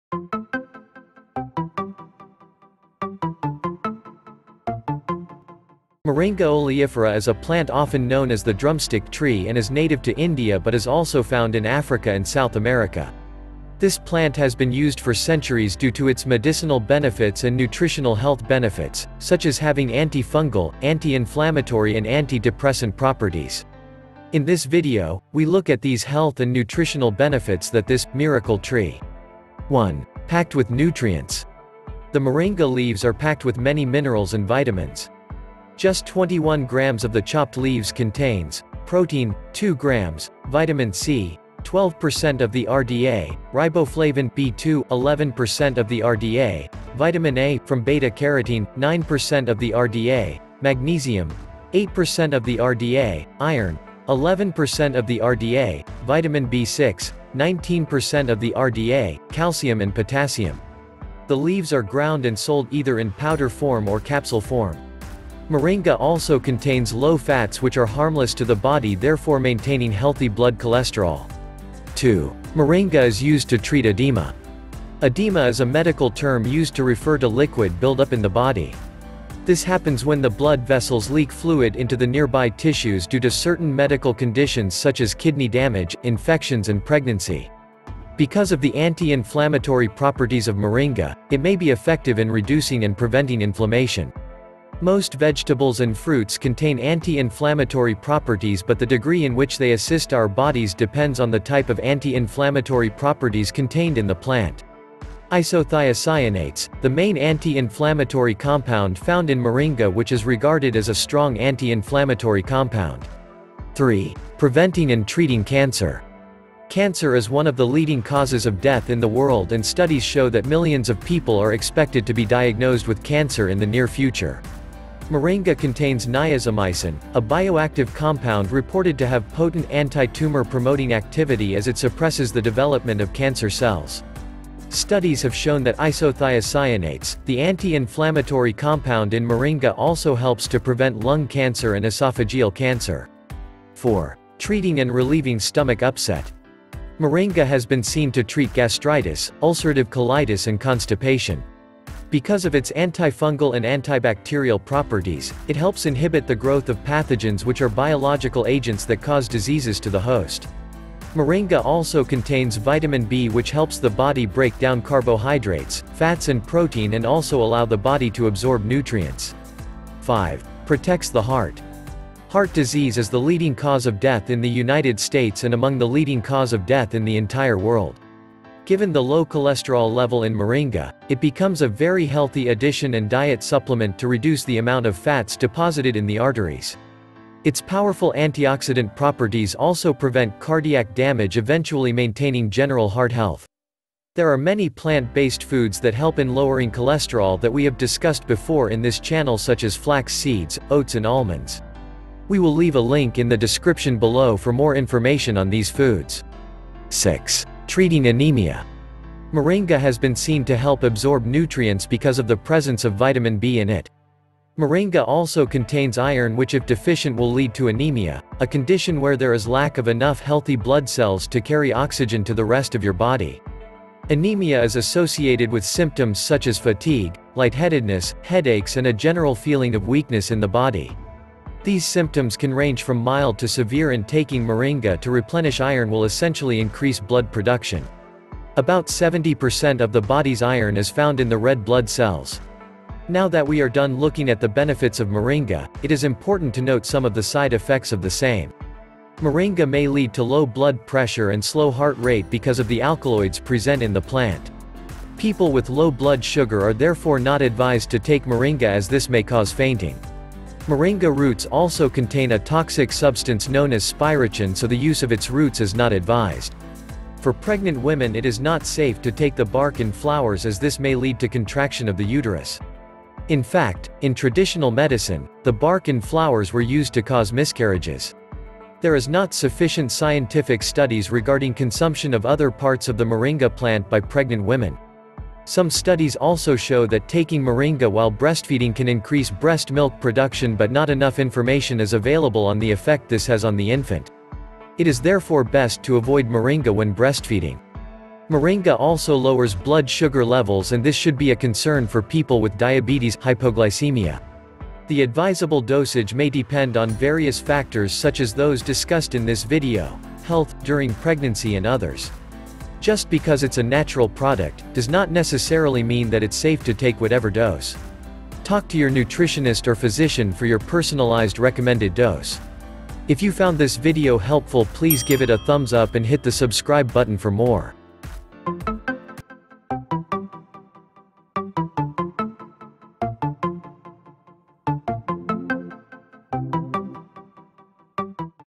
Health benefits of Moringa | Moringa Oleifera
In this video we look at 6 health benefits of moringa and some of the side effects as well.
Health benefits of moringa are;
1. Packed with nutrients.
The moringa leaves are packed with many minerals and vitamins
Moringa also contains low fats which are harmless to the body therefore maintaining healthy blood cholesterol.
2. Moringa is used to treat edema.
Because of the anti inflammatory properties of Moringa, it may be effective in reducing and preventing inflammation.
3. Preventing and treating Cancer.
Moringa contains Niazimicin, a bioactive compound reported to have potent antitumor promoting activity as it suppresses the development of cancer cells.
Studies have shown that isothiocyanates the anti inflammatory compound in Moringa also helps to prevent lung cancer and esophageal cancer.

4. Treating and relieving stomach upset.
Moringa has been seen to treat gastritis, ulcerative colitis and constipation.
Because of its antifungal and antibacterial properties, it helps inhibit the growth of pathogens which are biological agents that cause diseases to the host.
Moringa also contains vitamin B which helps the body break down carbohydrates, fats and protein and also allow the body to absorb nutrients.

5. Protects the Heart.
Given the low cholesterol level in Moringa, it becomes a very healthy addition and diet supplement to reduce the amount of fats deposited in the arteries.
Its powerful antioxidant properties also prevent cardiac damage eventually maintaining general heart health.
6. Treating anemia.
Moringa has been seen to help absorb nutrients because of the presence of vitamin b in it. Moringa also contains iron which if deficient will lead to anemia, a condition where there is lack of enough healthy blood cells to carry oxygen to the rest of your body.

TIMECODES:
0:00 Intro
0:30 Packed with nutrients.
1:12 Moringa is used to treat edema
1:54 Preventing and treating Cancer
2:24 Treating and relieving stomach upset
2:50 Protects the Heart
3:27 Treating anemia
4:05 Side effects